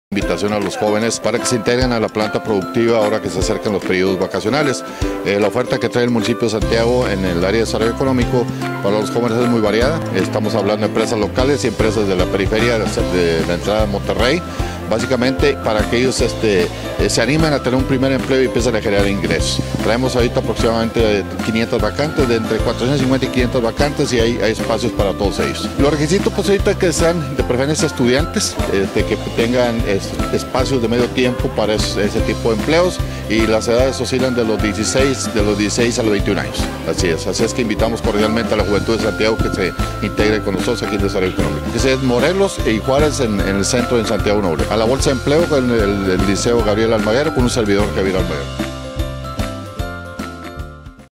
OFRECEN BOLSA DE TRABAJO A JOVENES ESTUDIANTES
Santiago.-  El Secretario de Desarrollo Económico; Javier Almaguer, hace una invitación a los jóvenes estudiantes para que se integren a la planta productiva  en los días vacacionales , con una gran oferta que cuenta el Municipio ya que cuenta con empresas locales y la periferia de la entrada a Monterrey.
Aproximadamente tienen entre 450 a 500 vacantes, los requisitos solo ser de preferencia estudiantes y cuenten con espacios de medios tiempos, con edades de 16 a 21 años.
Para cualquier duda o más información acudir a las oficinas de bolsa de empleo en Calle Morelos y Juárez, Centro de Santiago N.L.